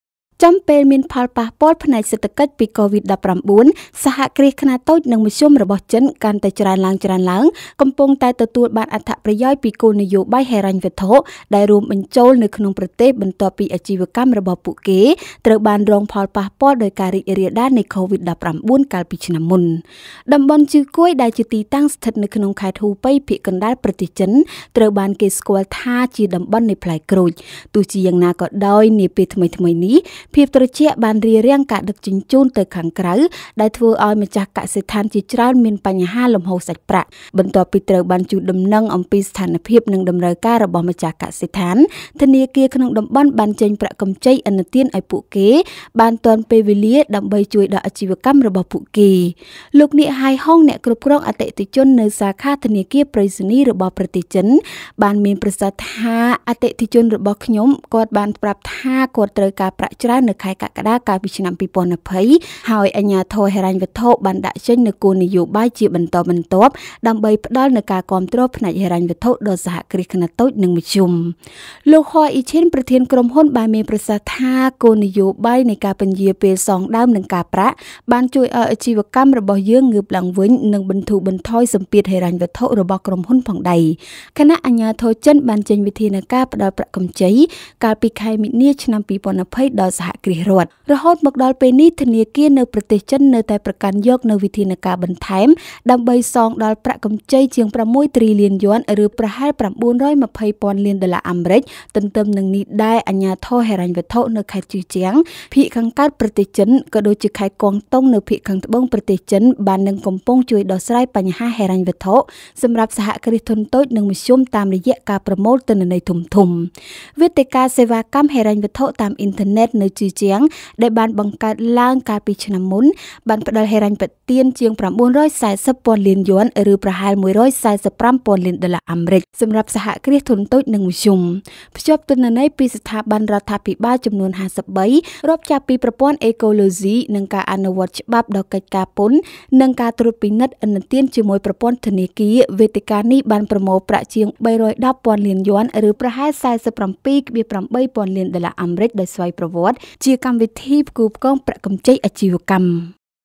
គោលនយោបាយហិរញ្ញវត្ថុជួយសហគ្រាសធុនតូចនិងមធ្យមធ្វើអាជីវកម្មឡើងវិញ
ចិន៖ ចំពេលមានផលប៉ះពាល់ផ្នែកសេដ្ឋកិច្ចពីកូវីដ១៩ សហគ្រាសខ្នាតតូចនិងមធ្យមរបស់ចិន កាន់តែច្រើនឡើងៗ កំពុងតែទទួលបានអត្ថប្រយោជន៍ពីគោលនយោបាយហិរញ្ញវត្ថុ ដែលរួមបញ្ចូលនៅក្នុងប្រទេស បន្ទាប់ពីអាជីវកម្មរបស់ពួកគេត្រូវបានរងផលប៉ះពាល់ដោយការរីករាលដាលនៃកូវីដ១៩ កាលពីឆ្នាំមុន។